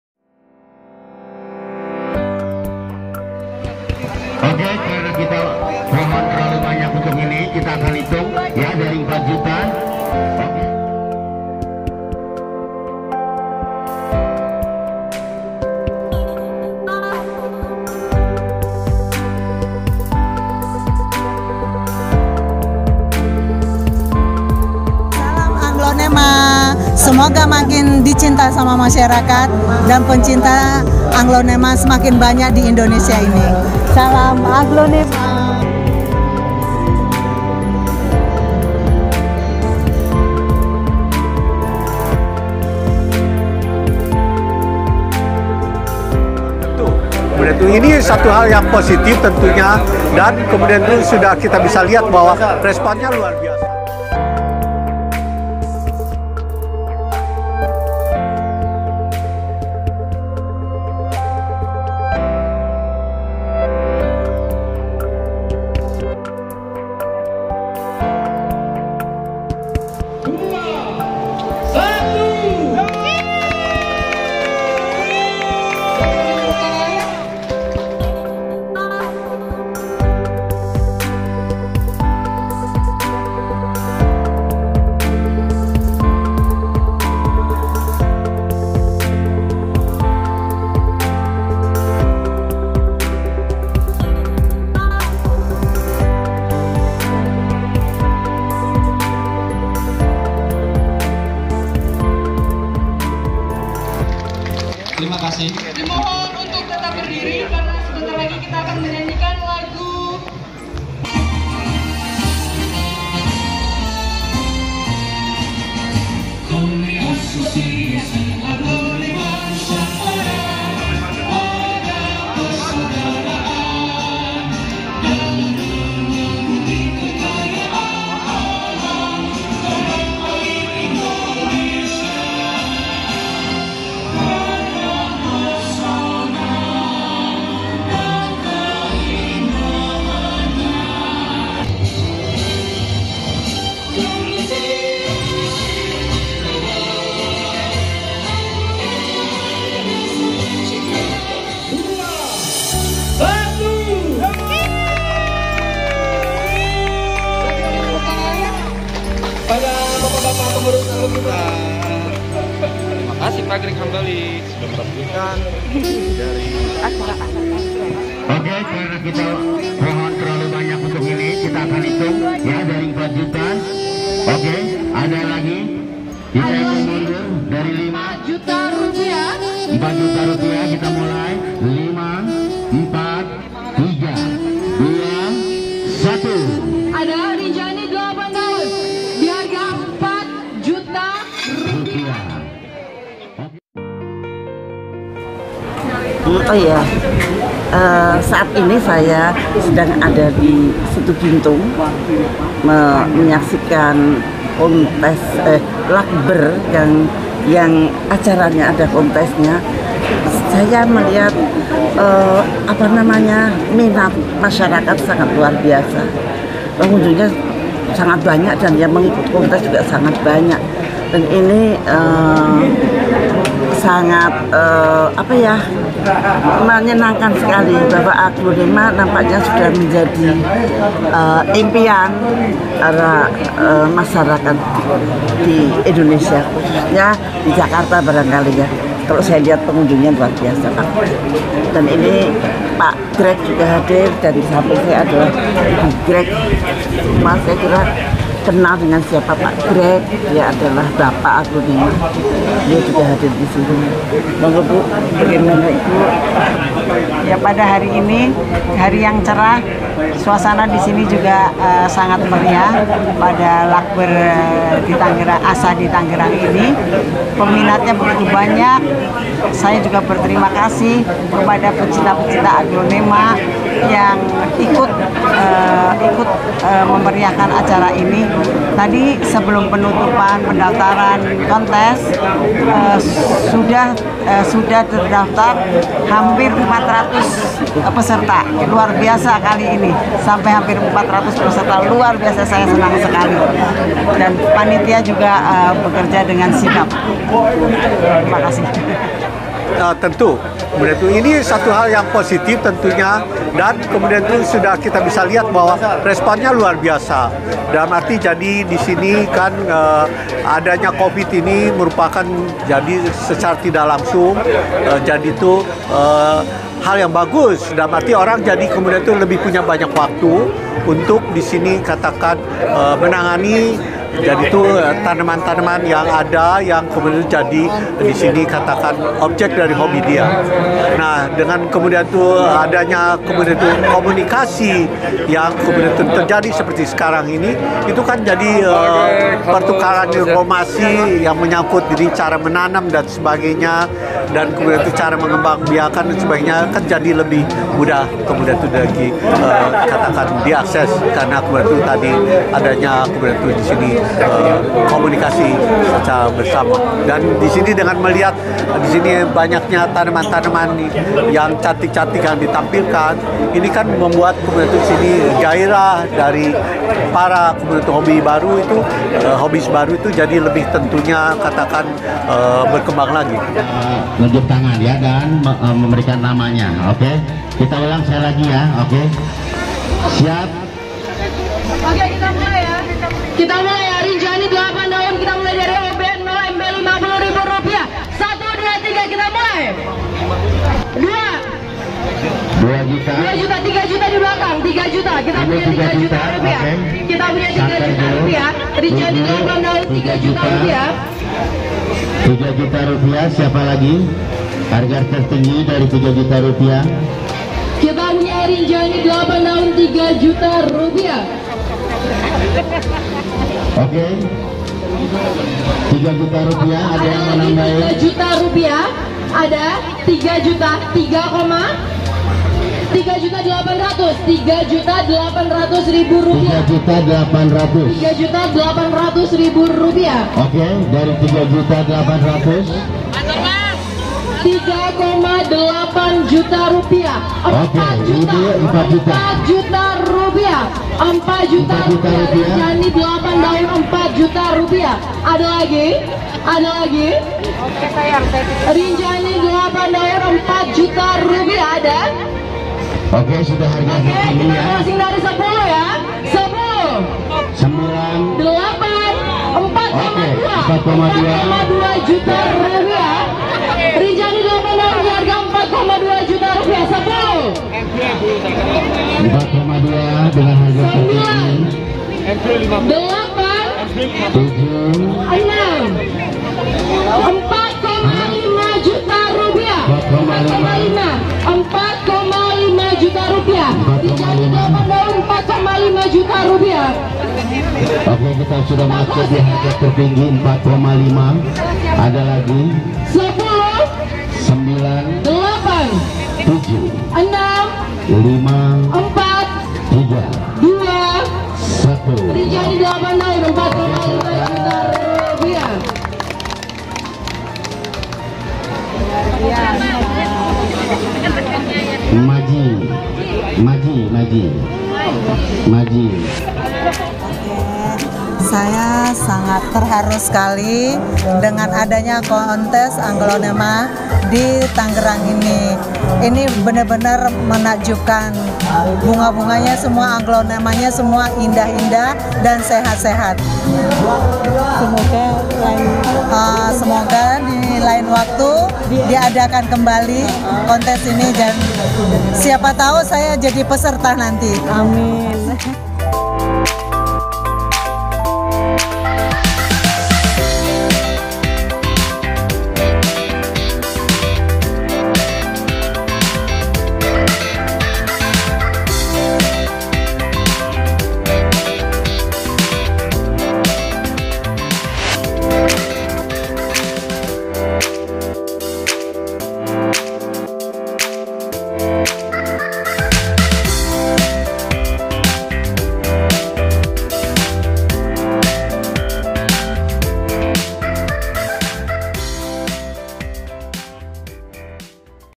LATBER ASA Tangerang Raya [Minggu, 20 Maret 2022] di Taman Wisata Situ Gintung
LATBER ASA (ASOSIASI AGLONEMA NUSANTARA) TANGERANG RAYA

Di Taman Wisata Pulau Situ Gintung - Ciputat Raya.

Yg telah di selenggarakan pada hari : Minggu, 20 Maret 2022

Telah di hadiri oleh maestro-maestro yaitu :

1. Bpk. Greg Hambali (Bpk. Maestro Aglonema).
2. Ibu Indri (Istri Bpk. Greg Hambali).
3. Ibu Hj. Dr. dr. Siti Fadilah Supari Sp. PJ (K) [Mantan Menteri Kesehatan].
4. Bang Azis Gagap.
5. Ketua ASA Pusat.
6. Para Penghoby Aglonema
7. Dan Masyarakat Lainnya.

Cp : Lana Flora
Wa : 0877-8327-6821